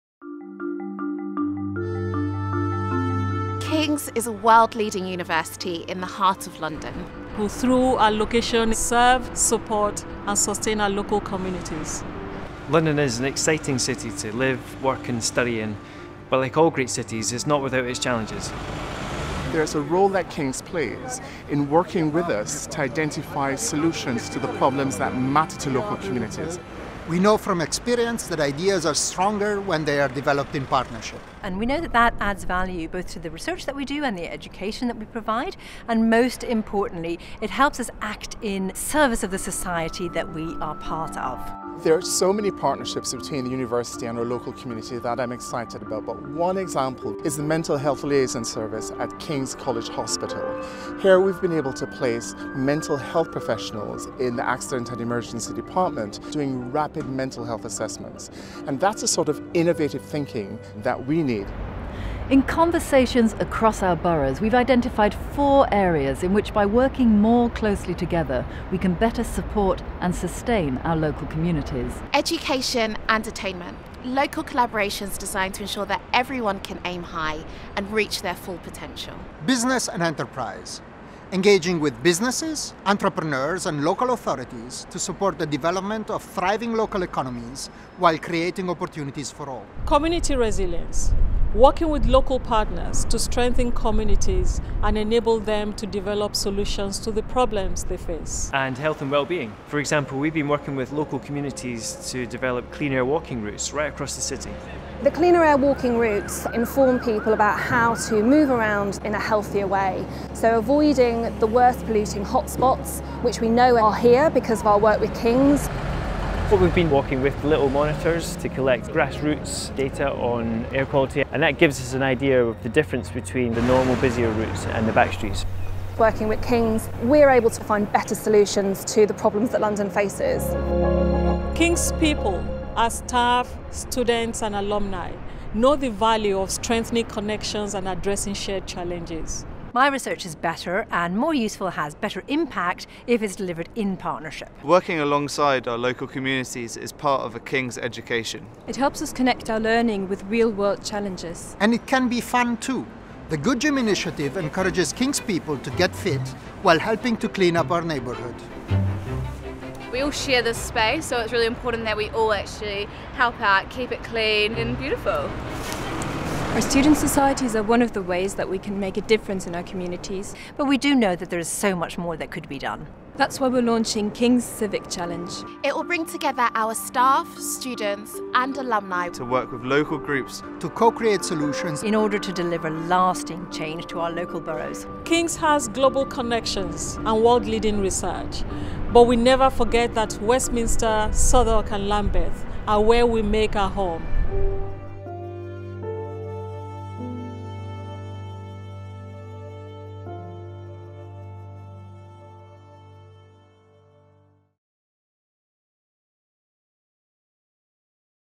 King’s celebrates local partnerships and launches new Civic Challenge
King’s Vision 2029 sets out five priorities for the university: research, education and service, played out in the context of our strong international connections and our location in the heart of London.

A new annual competition, King’s Civic Challenge, will empower students, staff and local communities to team up to co-create imaginative solutions to local challenges that will deliver lasting change within our communities.